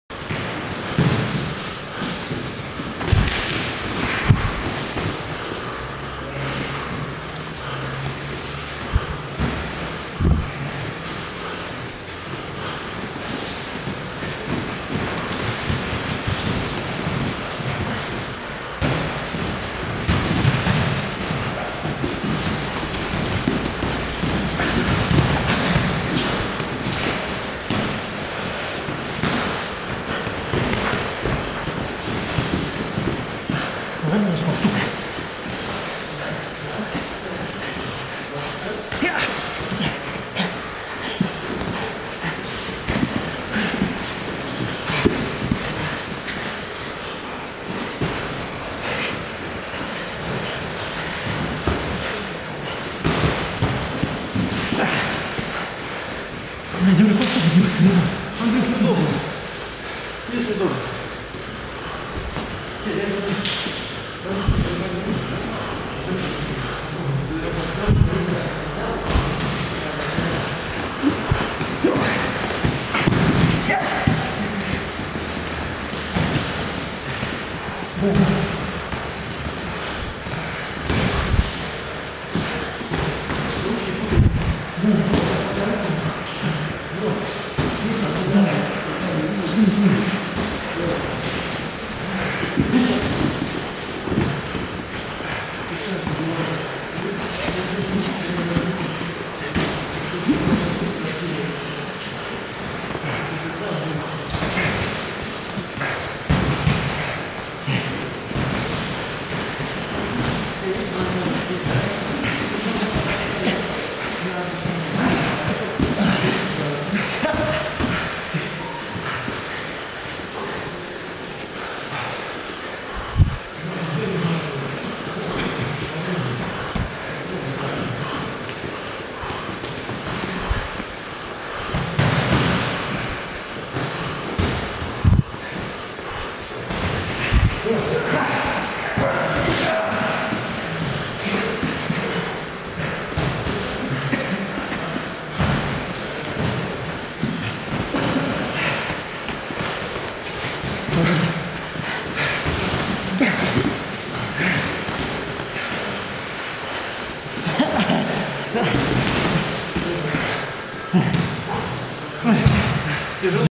МСМК России по самбо Дмитрий Минаков работа по заданию
Брянская школа микстфайта , МСМК России по самбо Дмитрий Минаков, работа по заданию, перевод в партер и проведение болевого или удушающего приема